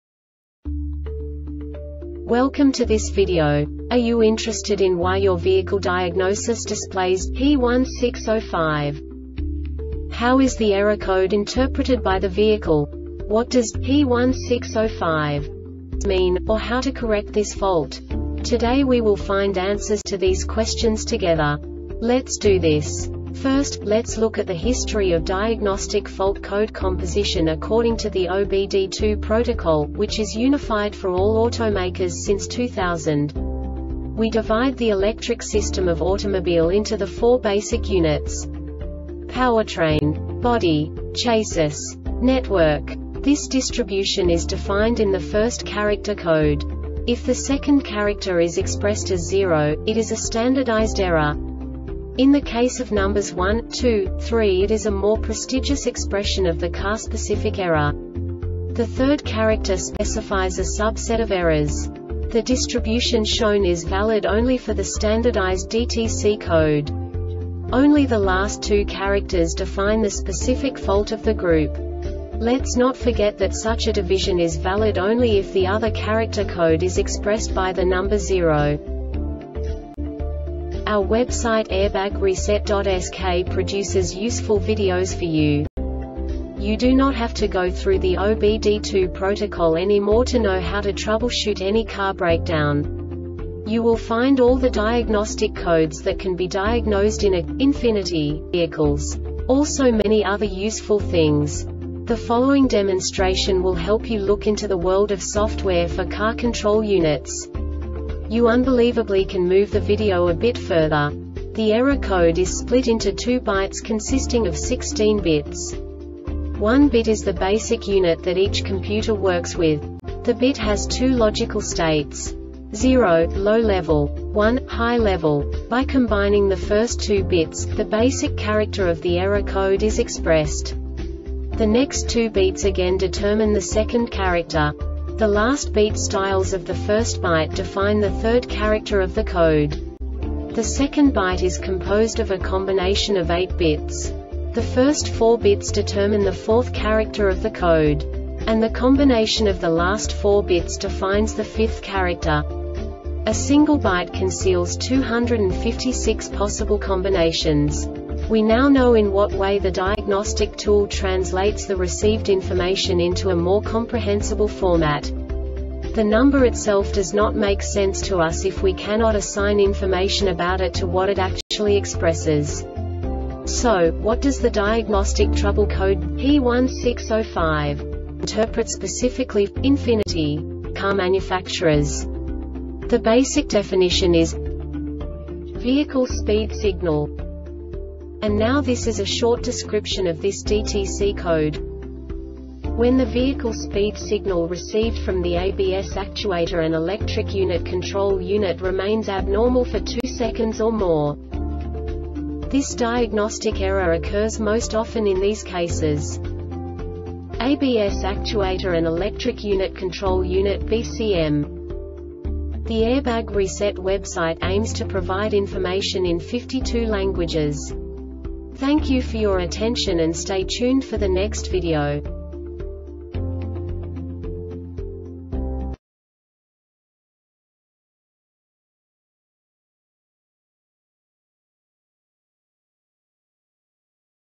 DTC Ford P1605 Short Explanation
Contents:
0:21 Basic DTC analysis according to OBD2 protocol standard.
1:48 Insight into programming
2:58 A diagnostically understandable description of the Diagnostic Trouble Code
3:05 Basic error definition

Make:
Ford
Code:
P1605
Definition:
Vehicle Speed Signal
Description:
When the vehicle speed signal received from the ABS actuator and electric unit (control unit) remains abnormal for 2 seconds or more.
Cause:
 ABS actuator and electric unit (control unit) BCM